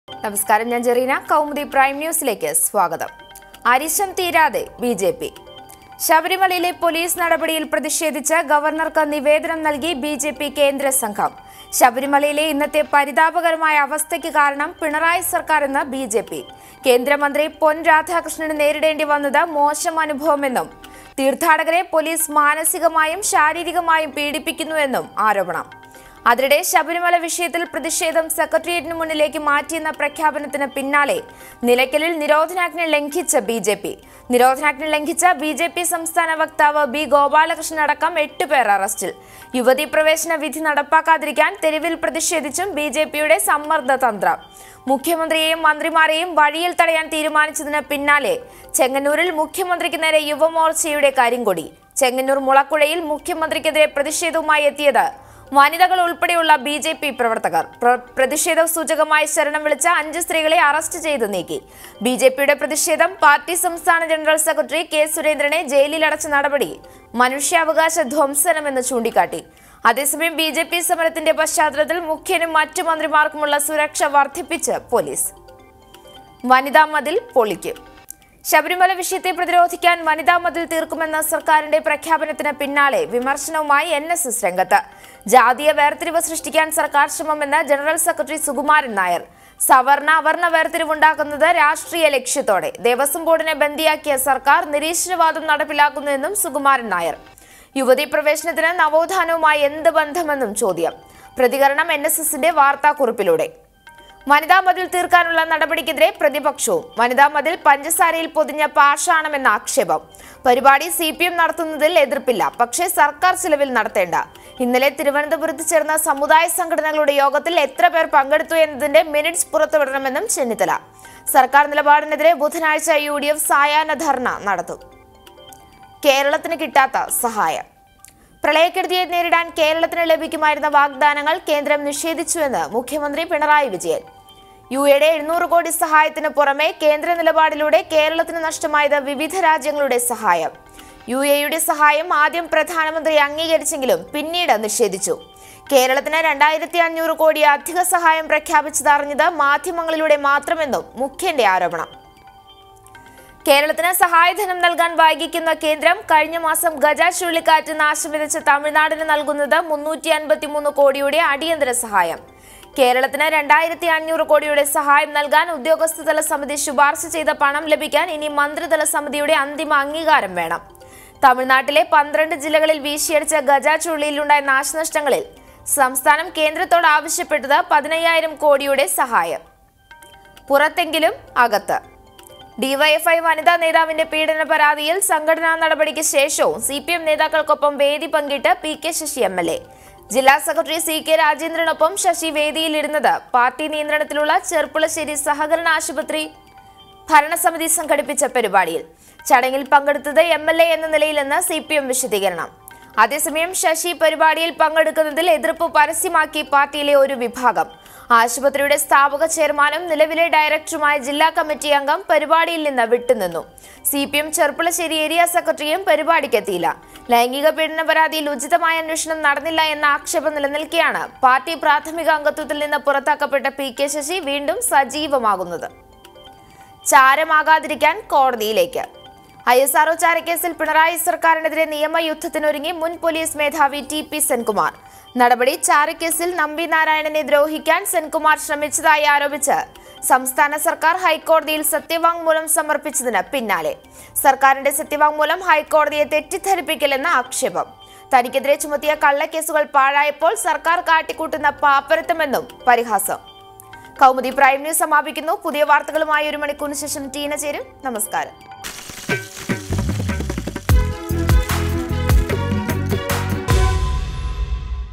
ഐ.എസ്.ആര്‍.ഒ ചാര കേസില്‍ പിണറായി സര്‍ക്കാരിന് എതിരെ നിയമ യുദ്ധത്തിന് ഒരുങ്ങി ടി.പി സെന്‍കുമാര്‍
1. ശബരിമലയിലെ പൊലീസ് നടപടിയില്‍ പ്രതിഷേധിച്ച് ഗവര്‍ണര്‍ക്ക് നിവേദനം നല്‍കി ബി.ജെ.പി കേന്ദ്ര സംഘം. ശബരിമലയിലെ ഇന്നത്തെ പരിതാപകരമായ അവസ്ഥക്ക് കാരണം പിണറായി സര്‍ക്കാര്‍ എന്ന് ബി.ജെ.പി. കേന്ദ്ര മന്ത്രി പൊന്‍ രാധാകൃഷ്ണന് നേരിടേണ്ടി വന്നത് മോശം അനുഭവം എന്നും തീര്‍ത്ഥാടകരെ പൊലീസ് മാനസികമായും ശാരീരികമായും പീഡിപ്പിക്കുന്നു എന്നും ആരോപണം
2. അതിനിടെ, ശബരിമല വിഷയത്തില്‍ പ്രതിഷേധം സെക്രട്ടറിയേറ്റിനു മുന്നിലേക്കു മാറ്റിയെന്ന പ്രഖ്യാപനത്തിന് പിന്നാലെ നിലയ്ക്കലില്‍ നിരോധനാജ്ഞ ലംഘിച്ച് ബി.ജെ.പി. നിരോധനാജ്ഞ ലംഘിച്ച ബി.ജെ.പി സംസ്ഥാന വക്താവ് ബി. ഗോപാലകൃഷ്ണന്‍ അടക്കം എട്ടു പേര്‍ അറസ്റ്റില്‍. യുവതീ പ്രവേശന വിധി നടപ്പാക്കാതിരിക്കാന്‍ തെരുവില്‍ പ്രതിഷേധിച്ചും ബി.ജെ.പിയുടെ സമ്മര്‍ദ്ദ തന്ത്രം. മുഖ്യമന്ത്രിയേയും മന്ത്രിമാരേയും വഴിയില്‍ തടയാന്‍ തീരുമാനിച്ചതിനു പിന്നാലെ, ചെങ്ങന്നൂരില്‍ മുഖ്യമന്ത്രിക്കു നേരെ  യുവമോര്‍ച്ചയുടെ കരിങ്കൊടി
3. ചെങ്ങന്നൂര്‍ മുളക്കുഴയില്‍ മുഖ്യമന്ത്രിക്ക് എതിരെ പ്രതിഷേധവുമായി എത്തിയത് വനിതകള്‍ ഉള്‍പ്പെടെ ഉള്ള ബി.ജെ.പി പ്രവര്‍ത്തകര്‍. പ്രതിഷേധ സൂചകമായി ശരണം വിളിച്ച അഞ്ച് സ്ത്രീകളെ അറസ്റ്റ് ചെയ്തു നീക്കി. ബി.ജെ.പിയുടെ പ്രതിഷേധം, പാര്‍ട്ടി സംസ്ഥാന ജനറല്‍ സെക്രട്ടറി കെ.സുരേന്ദ്രനെ ജയിലില്‍ അടച്ച നടപടി മനുഷ്യാവകാശ ധ്വംസനം എന്ന് ചൂണ്ടിക്കാട്ടി. അതേസമയം, ബി.ജെ.പി സമരത്തിന്റെ പശ്ചാത്തലത്തില്‍ മുഖ്യനും മറ്റു മന്ത്രിമാര്‍ക്കും ഉള്ള സുരക്ഷ വര്‍ദ്ധിപ്പിച്ച് പൊലീസ്.
4. ശബരിമല വിഷയത്തെ പ്രതിരോധിക്കാന്‍ വനിതാ മതില്‍ തീര്‍ക്കും എന്ന സര്‍ക്കാരിന്റെ പ്രഖ്യാപനത്തിന് പിന്നാലെ വിമര്‍ശനവുമായി എന്‍.എസ്.എസ് രംഗത്ത്. ജാതീയ വേര്‍തിരിവ് സൃഷ്ടിക്കാന്‍ സര്‍ക്കാര്‍ ശ്രമം എന്ന് ജനറല്‍ സെക്രട്ടറി സുകുമാരന്‍ നായര്‍. സവര്‍ണ - അവര്‍ണ വേര്‍തിരിവ് ഉണ്ടാക്കുന്നത് രാഷ്ട്രീയ ലക്ഷ്യത്തോടെ. ദേവസ്വം ബോര്‍ഡിനെ ബന്ദിയാക്കിയ സര്‍ക്കാര്‍, നിരീശ്വര വാദം നടപ്പിലാക്കുന്നു എന്നും സുകുമാരന്‍ നായര്‍. യുവതീ പ്രവേശനത്തിന് നവോത്ഥാനവുമായി എന്ത് ബന്ധം എന്നും ചോദ്യം. പ്രതികരണം എന്‍.എസ്.എസിന്റെ വാര്‍ത്താക്കുറിപ്പിലൂടെ
5. വനിതാ മതില്‍ തീര്‍ക്കാനുള്ള നടപടിക്ക് എതിരെ പ്രതിപക്ഷവും. വനിതാ മതില്‍ പഞ്ചസാരയില്‍ പൊതിഞ്ഞ പാഷണം എന്ന് ആക്ഷേപം. പരിപാടി സി.പി.എം നടത്തുന്നതില്‍ എതിര്‍പ്പില്ല- പക്ഷേ സര്‍ക്കാര്‍ ചിലവില്‍ നടത്തേണ്ട. ഇന്നലെ തിരുവനന്തപുരത്ത് ചേര്‍ന്ന സമുദായ സംഘടനകളുടെ യോഗത്തില്‍ എത്ര പേര്‍ പങ്കെടുത്തു എന്നതിന്റെ മിനിട്സ് പുറത്തു വിടണം എന്നും ചെന്നിത്തല. സര്‍ക്കാര്‍ നിലപാടിന് എതിരെ ബുധനാഴ്ച യു.ഡി.എഫ് സായാഹ്ന ധര്‍ണ നടത്തും
6. പ്രളയക്കെടുതിയെ നേരിടാന്‍ കേരളത്തിന് ലഭിക്കുമായിരുന്ന വാഗ്ദാനങ്ങള്‍ കേന്ദ്രം നിഷേധിച്ചു എന്ന് മുഖ്യമന്ത്രി പിണറായി വിജയന്‍. യു.എ.ഇയുടെ 700 കോടി സഹായത്തിന് പുറമെ കേന്ദ്ര നിലപാടിലൂടെ കേരളത്തിന് നഷ്ടമായത് വിവിധ രാജ്യങ്ങളുടെ സഹായം. യു.എ.ഇയുടെ സഹായം ആദ്യം പ്രധാനമന്ത്രി അംഗീകരിച്ചെങ്കിലും പിന്നീട് നിഷേധിച്ചു. കേരളത്തിന് 2500 കോടി അധിക സഹായം പ്രഖ്യാപിച്ചത് അറിഞ്ഞത് മാദ്ധ്യമങ്ങളിലൂടെ മാത്രം എന്നും മുഖ്യന്റെ ആരോപണം
7. കേരളത്തിന് സഹായധനം നല്‍കാന്‍ വൈകിക്കുന്ന കേന്ദ്രം, കഴിഞ്ഞ മാസം ഗജ ചുഴലിക്കാറ്റ് നാശം വിതച്ച തമിഴ്നാടിന് നല്‍കുന്നത് 353 കോടിയുടെ അടിയന്തര സഹായം. കേരളത്തിന് 2500 കോടിയുടെ സഹായം നല്‍കാന്‍ ഉദ്യോഗസ്ഥ തല സമിതി ശുപാര്‍ശ ചെയ്ത പണം ലഭിക്കാന്‍ ഇനി മന്ത്രിതല സമിതിയുടെ അന്തിമ അംഗീകാരം വേണം. തമിഴ്നാട്ടിലെ 12 ജില്ലകളില്‍ വീശിയടിച്ച ഗജ ചുഴലിയില്‍ ഉണ്ടായ നാശനഷ്ടങ്ങളില്‍ സംസ്ഥാനം കേന്ദ്രത്തോട് ആവശ്യപ്പെട്ടത് 15,000 കോടിയുടെ സഹാം.
8. ഡി.വൈ.എഫ്.ഐ വനിതാ നേതാവിന്റെ പീഡന പരാതിയില്‍ സംഘടനാ നടപടിക്ക് ശേഷവും സി.പി.എം നേതാക്കള്‍ക്ക് ഒപ്പം വേദി പങ്കിട്ട് പി.കെ ശശി എം.എല്‍.എ. ജില്ലാ സെക്രട്ടറി സി.കെ രാജേന്ദ്രന് ഒപ്പം ശശി വേദിയില്‍ ഇരുന്നത്, പാര്‍ട്ടി നിയന്ത്രണത്തിലുള്ള ചെര്‍പ്പുളശ്ശരേി സഹകരണ ആശുപത്രി ഭരണ സമിതി സംഘടിപ്പിച്ച പരിപാടയില്‍. ചടങ്ങില്‍  പങ്കെടുത്തത് എം.എല്‍.എ എന്ന നിലയില്‍ എന്ന് സി.പി.എം വിശദീകരണം
9. അതേസമയം, ശശി പരിപാടിയില്‍ പങ്കെടുക്കുന്നതില്‍ എതിര്‍പ്പ് പരസ്യമാക്കി പാര്‍ട്ടിയിലെ ഒരു വിഭാഗം. ആശുപത്രിയുടെ സ്ഥാപക ചെയര്‍മാനും നിലവിലെ ഡയറക്ടറും ആയ ജില്ലാ കമ്മിറ്റി അംഗം പരിപാടിയില്‍ നിന്ന് വിട്ടു നിന്നു. സി.പി.എം ചെര്‍പ്പുളശ്ശേരി ഏരിയാ സെക്രട്ടറിയും പരിപാടിക്ക് എത്തിയില്ല. ലൈംഗിക പീഡന പരാതിയില്‍ ഉചിതമായ അന്വേഷണം നടന്നില്ല എന്ന ആക്ഷേപം നിലനില്‍ക്കെ ആണ് പാര്‍ട്ടി പ്രാഥമിക അംഗത്വത്തില്‍ നിന്ന് പുറത്താക്കപ്പെട്ട പി.കെ ശശി വീണ്ടും സജീവം ആകുന്നത്
10. ഐ.എസ്.ആര്‍.ഒ ചാര കേസില്‍ പിണറായി സര്‍ക്കാരിന് എതിരെ നിയമ യുദ്ധത്തിന് ഒരുങ്ങി മുന്‍ പൊലീസ് മേധാവി ടി.പി സെന്‍കുമാര്‍. നടപടി, ചാരക്കേസില്‍ നമ്പി നാരായണനെ ദ്രോഹിക്കാന്‍ സെന്‍കുമാര്‍ ശ്രമിച്ചതായി  ആരോപിച്ച് സംസ്ഥാന സര്‍ക്കാര്‍ ഹൈക്കോടതിയില്‍ സത്യവാങ്മൂലം സമര്‍പ്പിച്ചതിന് പിന്നാലെ. സര്‍ക്കാരിന്റെ സത്യവാങ്മൂലം ഹൈക്കോടതിയെ തെറ്റിദ്ധരിപ്പിക്കല്‍ എന്ന് എന്ന് ആക്ഷേപം. തനിക്ക് എതിരെ ചുമത്തിയ കള്ളക്കേസുകള്‍ പാഴായപ്പോള്‍ സര്‍ക്കാര്‍ കാട്ടിക്കൂട്ടുന്ന പാപ്പരത്തം എന്നും പരിഹാസം